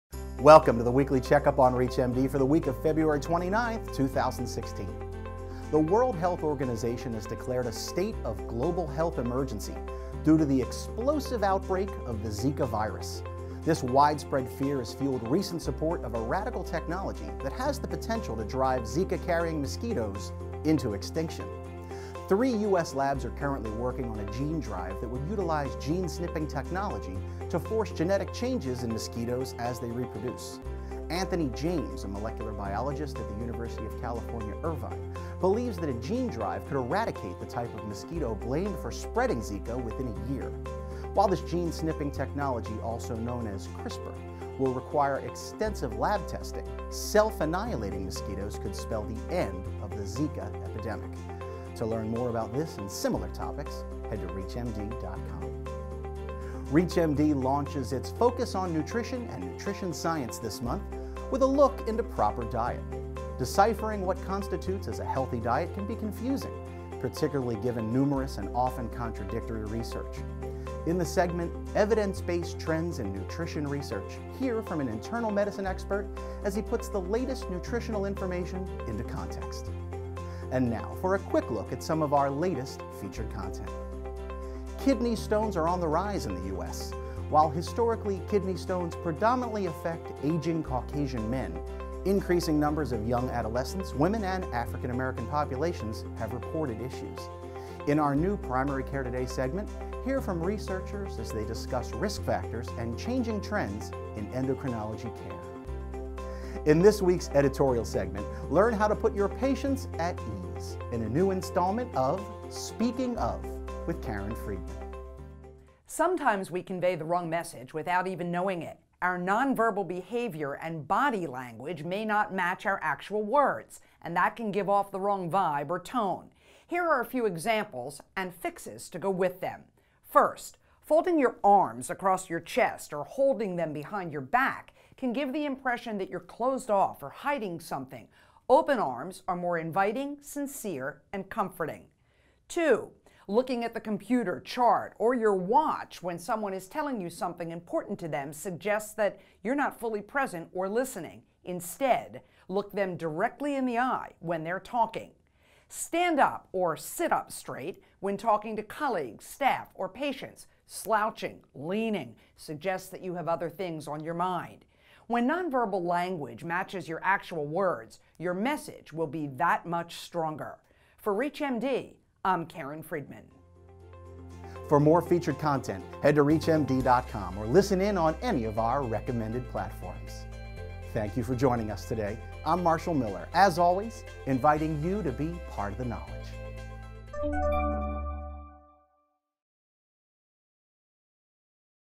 Weekly Check-up | February 29th 2016
Updates on the Zika Virus, trends in nutrition research, and the rise of kidney stones in children this week